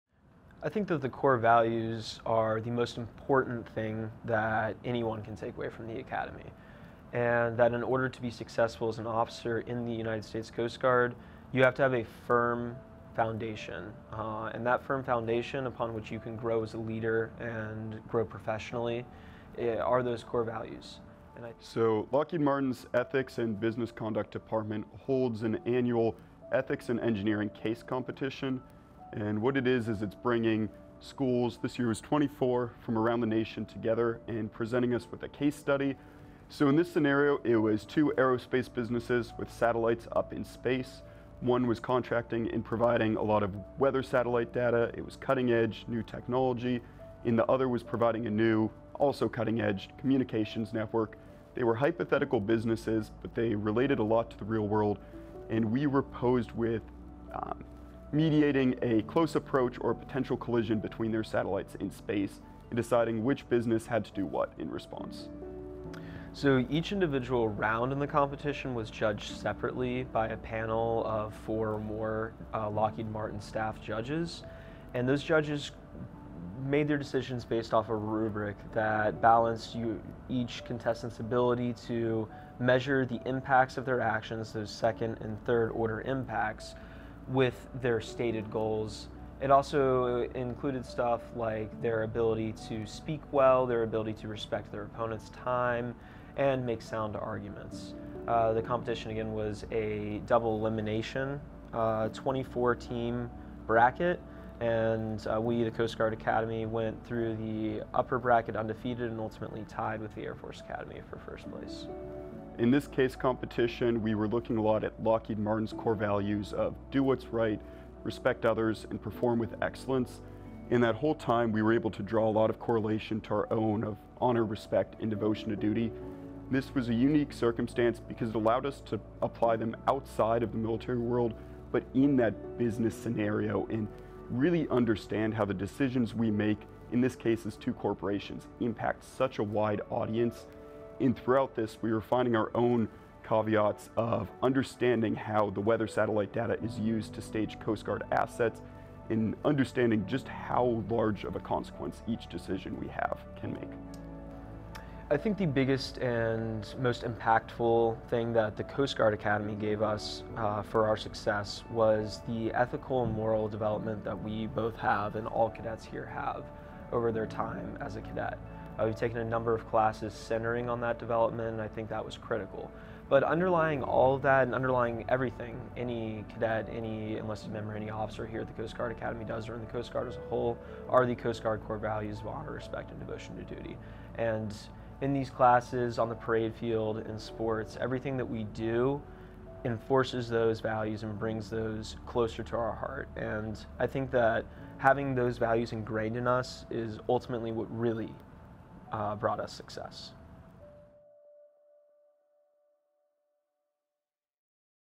Ethics in Engineering // CGA Wins Lockheed Martin's Case Competition
Congratulations to 1/c Jacob Schellman and Ryan Von Brock! These Electrical Engineers represented the Coast Guard at the Lockheed Martin's Ethics in Engineering Case Competition, earning first place with the U.S. Air Force Academy.

Faced with a (hypothetical) collision of two state-of-the-art satelites, the teams had to make decisions while weighing the ethical, business, and engineering consequences.

Both Schellman and Von Brock agreed that Lockheed Martin and the U.S. Coast Guard share similar core values, and that laid the foundation for their success.